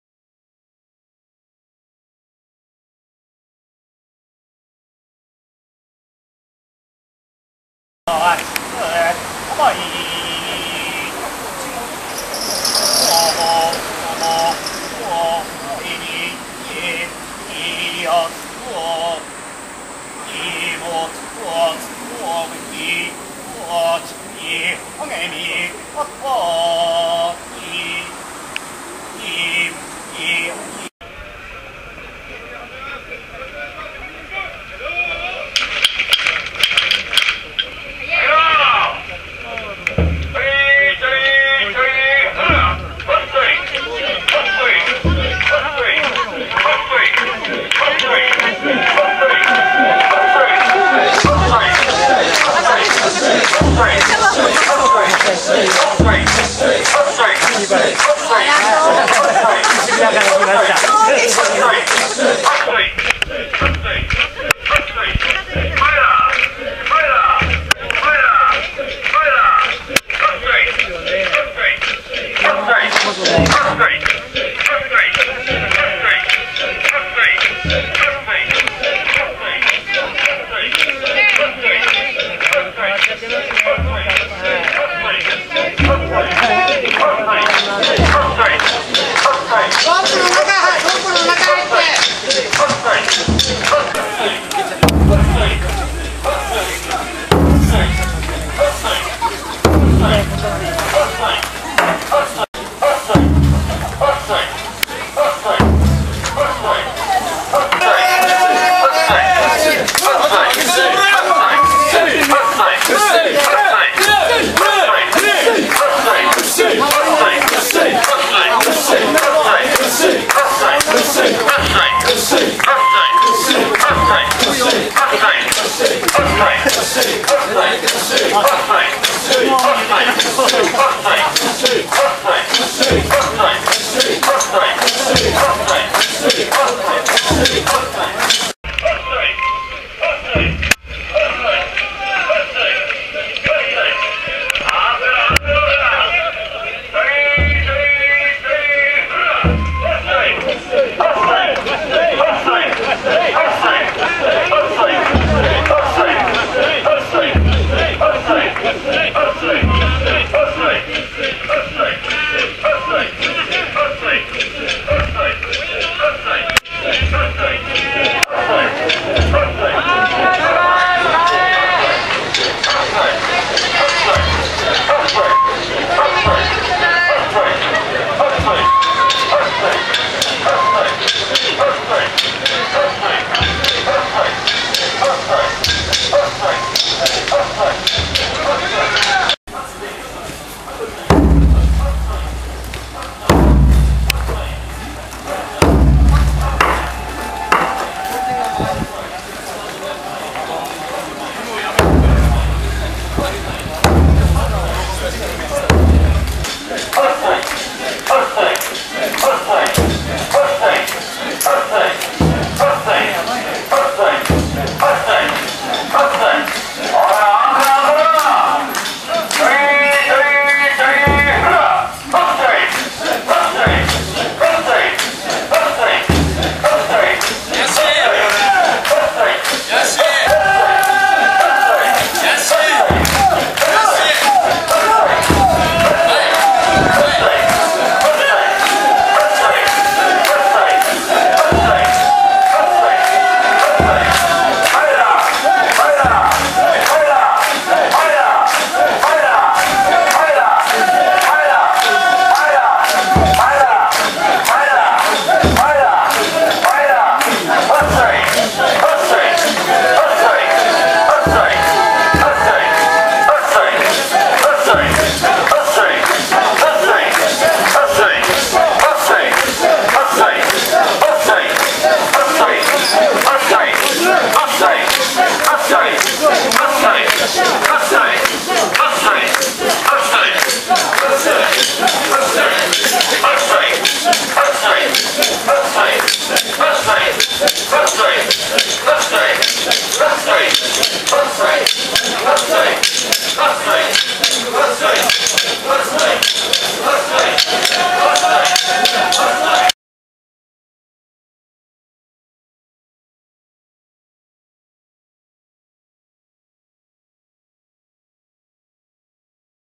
水海道・祇園祭2017年Ⅰ．神輿巡行
７月１５日（土）と１６日（日）、水海道・祇園祭がおこなわれました。